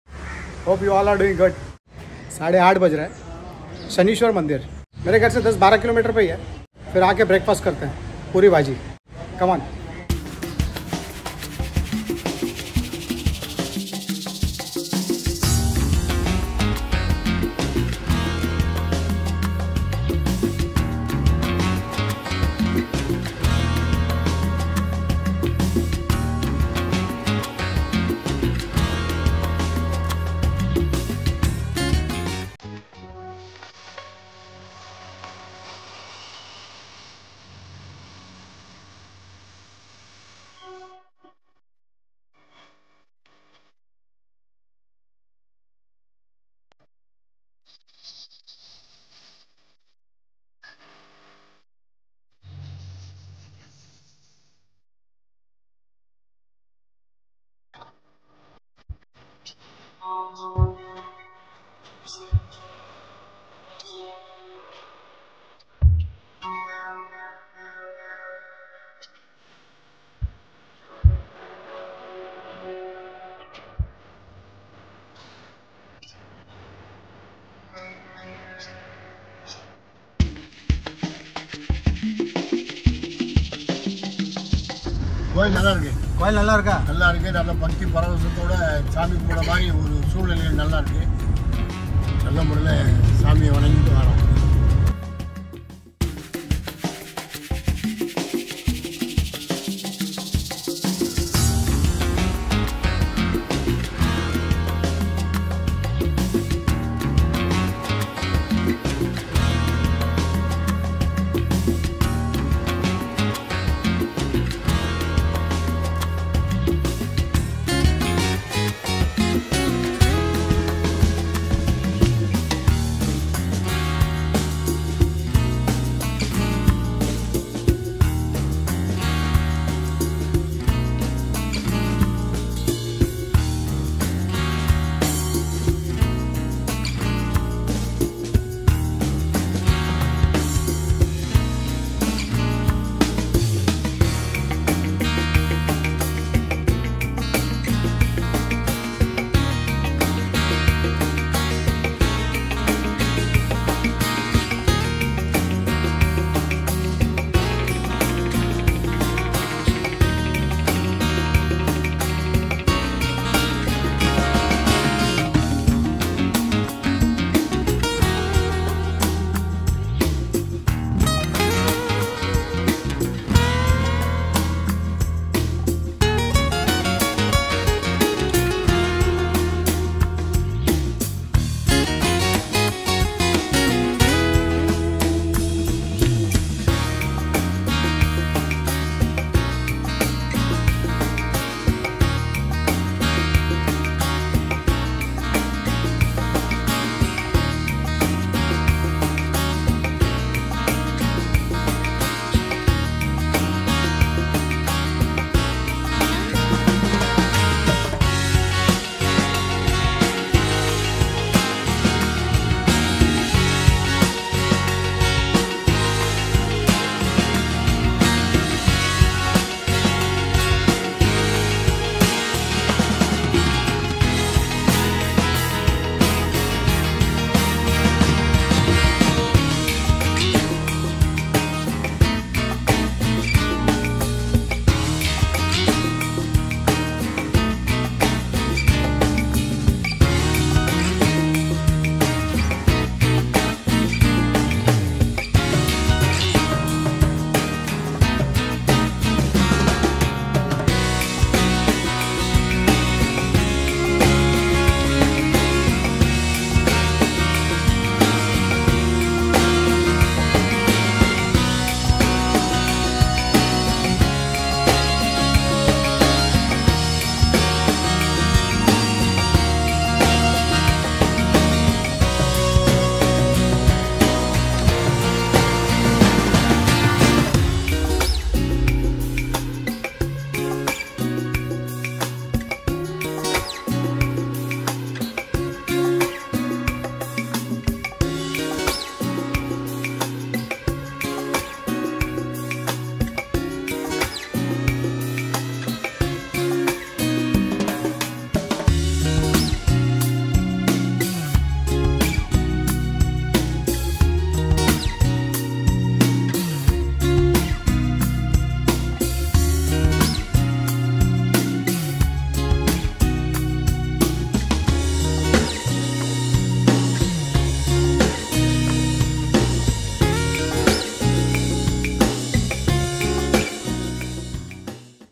Saturday Breakfast vlog - Big Size Poori with Soft Potato Bhaji | Poori Sabzi Recipe | Ammus Kitchen
How to make Poori Bhaji - An excellent breakfast for a lazy Saturday morning.A deep fried flaky bread called puris and medium spiced boiled potato curry called bhaji.. In India, puri bhaji gravy recipe is must for breakfast. Also puri bhaji gravy recipe can double up as a tiffin recipe.

Cuisine : Indian
Course : Snacks and Starters
Prep Time : 21-25 minutes
Cook time : 21-25 minutes
Serve : 4
Level Of Cooking : Easy
Taste : Spicy